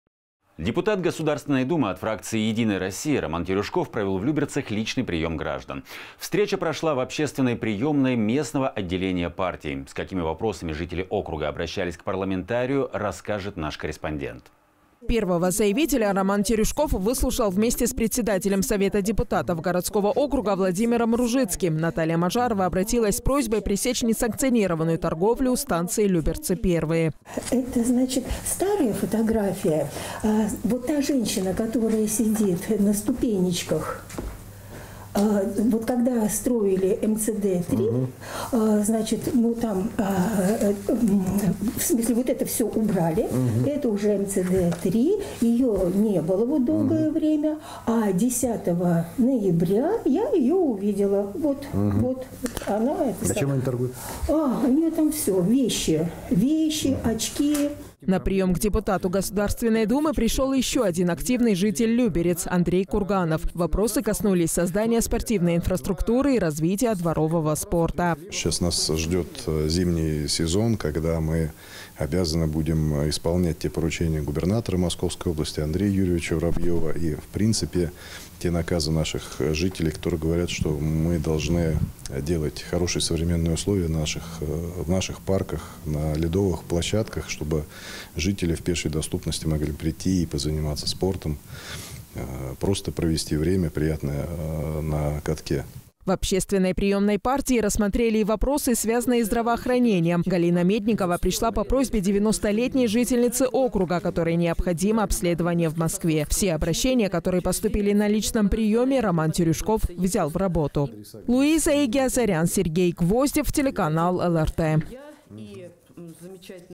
Роман Терюшков провел личный прием граждан
Депутат Государственной думы от фракции «Единая Россия»  Роман Терюшков провел в Люберцах личный прием граждан. Встреча прошла в общественной приемной местного отделения партии. С какими вопросами жители округа обратились к парламентарию, расскажет наш корреспондент.
Первого заявителя Роман Терюшков выслушал вместе с председателем Совета депутатов городского округа Владимиром Ружицким. Наталья Можарова обратилась с просьбой пресечь несанкционированную торговлю у станции Люберцы-1.
Наталья Можарова, жительница г.о. Люберцы:
«Это старые фотографии. Вон та женщина, которая сидит на ступенечках, когда строили МЦД -3, вот это все убрали, это уже МЦД-3, ее не было долгое время, а 10 ноября я ее увидела, вот она. У нее там все – вещи, очки».
На прием к депутату Государственной думы пришел еще один активный житель Люберец – Андрей Курганов. Вопросы коснулись создания спортивной инфраструктуры и развития дворового спорта.
Роман Терюшков, депутат Государственной думы РФ:
«Сейчас нас ждет зимний сезон, когда мы обязаны будем исполнять поручения губернатора Московской области Андрея Юрьевича Воробьева и, в принципе, те наказы наших жителей, которые говорят, что мы должны делать хорошие, современные условия в наших парках, ледовых площадках, чтобы жители в пешей доступности могли прийти и позаниматься спортом, просто провести время приятное на катке».
В общественной приемной партии рассмотрели и вопросы, связанные с здравоохранением. Галина Медникова пришла по просьбе 90-летней жительницы округа, которой необходимо обследование в Москве. Все обращения, которые поступили на личном приеме, Роман Терюшков взял в работу.